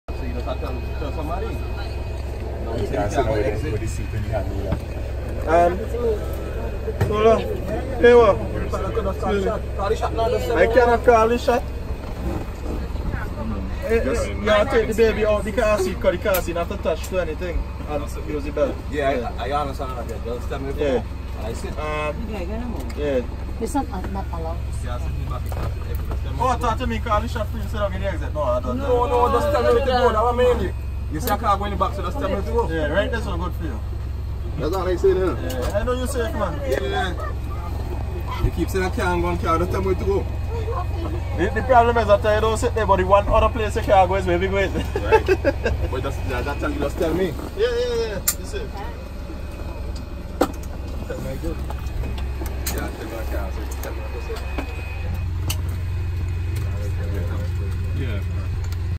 SVG Air from St Vincent to Union Island part 1
Boarding SVG at Argyle International Airpot